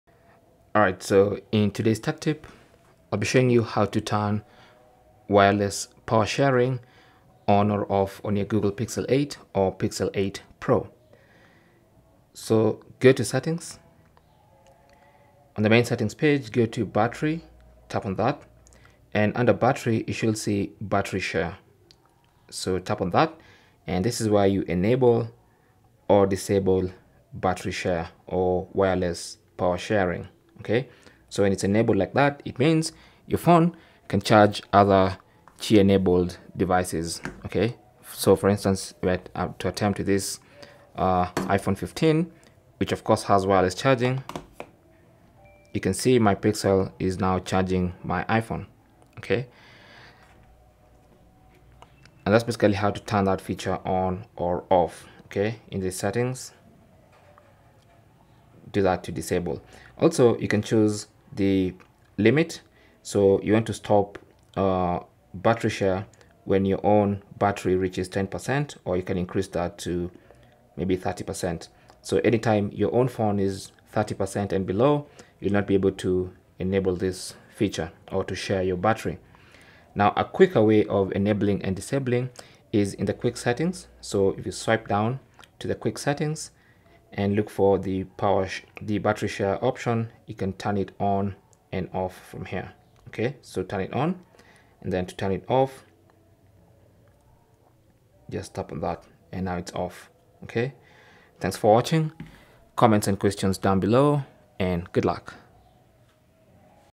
Google Pixel 8 / Pixel 8 Pro: How To Enable / Disable Battery Share
Learn How To Enable / Disable Battery On Share Google Pixel 8 & Pixel 8 Pro. In this video, we'll show you how to enable and disable Battery Share on your Google Pixel 8 Pro.

Battery Share is a fantastic feature that allows you to share your device's battery with other compatible devices, like your earbuds, smartwatches, or even a friend's phone. Whether you want to extend the battery life of your accessories or help out a friend in need, we've got you covered.

In this step-by-step tutorial, we'll walk you through the process, so you can make the most of this convenient feature. It's easy to do, and we'll cover all the important details, so you can use Battery Share confidently.

How To Enable Battery Share On Google Pixel 8 & Pixel 8 Pro

1. Open the Settings app. Scroll and select Battery.

2. Under Battery, locate the option Battery Share. Tap on it.

3. Tap on the toggle to turn it on. Then you should see a slider labelled Stop Sharing When Phone Reaches. This allows you to choose what percentage your phone will stop battery sharing.

How To Disable Battery Share On Google Pixel 8 & Pixel 8 Pro

1. Open the Settings app. Scroll and select Battery.

2. Under Battery, locate the option Battery Share. Tap on it.

3. Tap on the toggle to turn it off.

Cell Phone Tripod Adapter